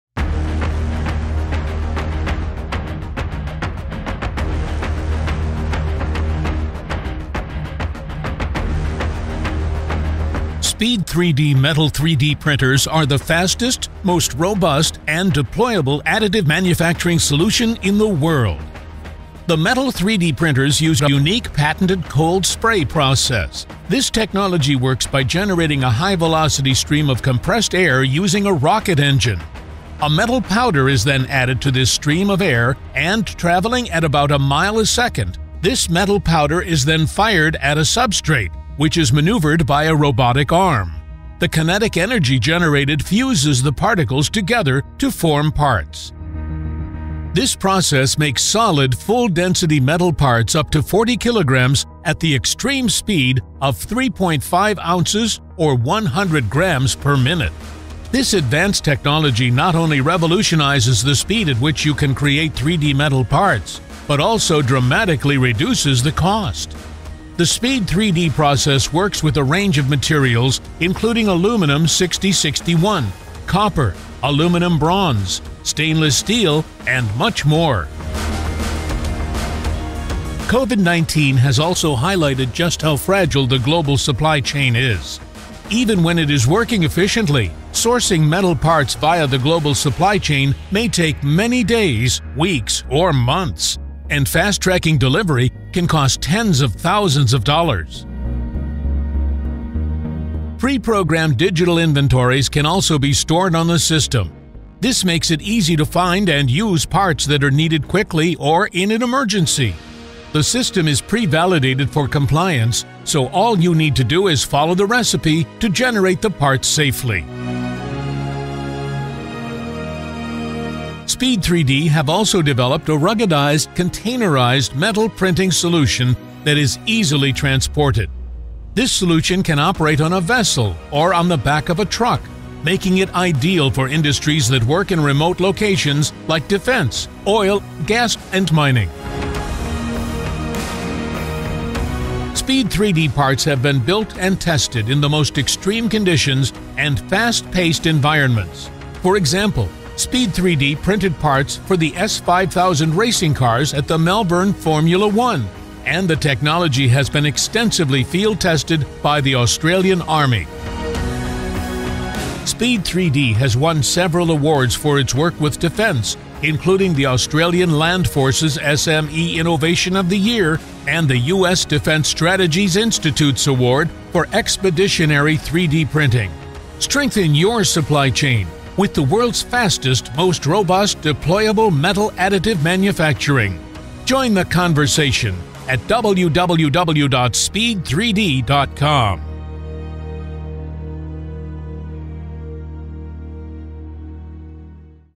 The World's Fastest Metal 3D Printers in the US
SPEE3D will be at the RAPID + TCT Conference in Detroit Michigan on May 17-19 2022, displaying their cold spray 3D printed wheel for automobiles, as well as the live manufacturing of copper battery terminals. We will be located at booth #1901!